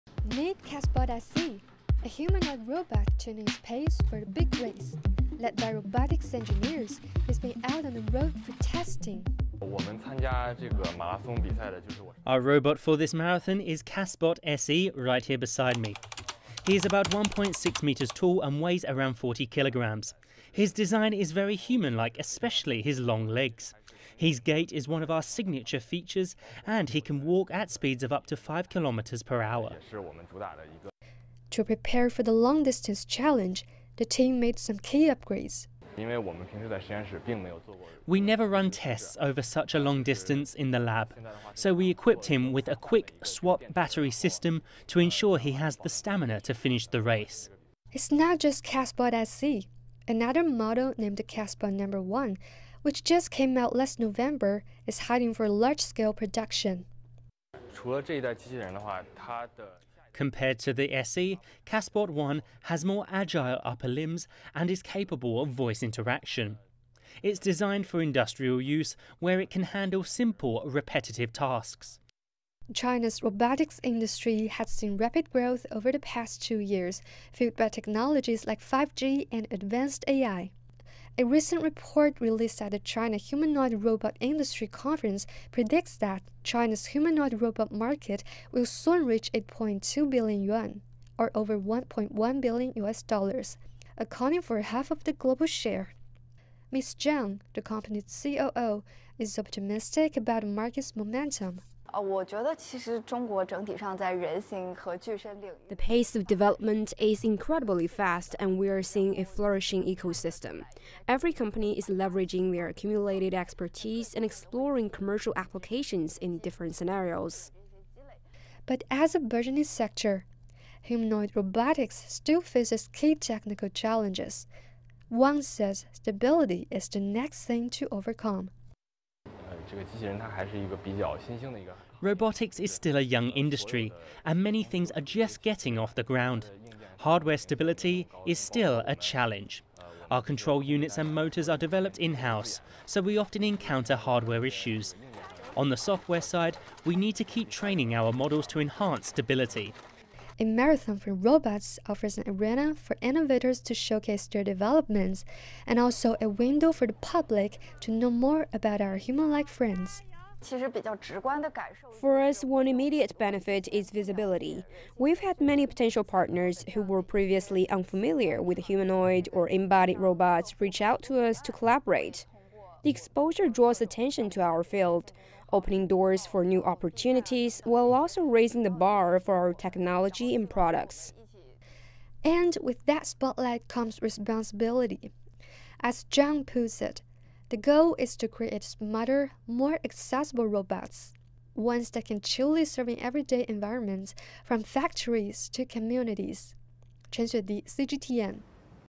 China showcases robotics innovation in world’s first 21-Km race with robot ‘runners’
Ahead of the world’s first 21-km race featuring robot "runners," CGTN took a close look at CASBOT SE, one of the robots that crossed the finish line. Its creators spent countless hours researching and training to make sure the robot was race-ready, underscoring China’s rapid advancements in robotics.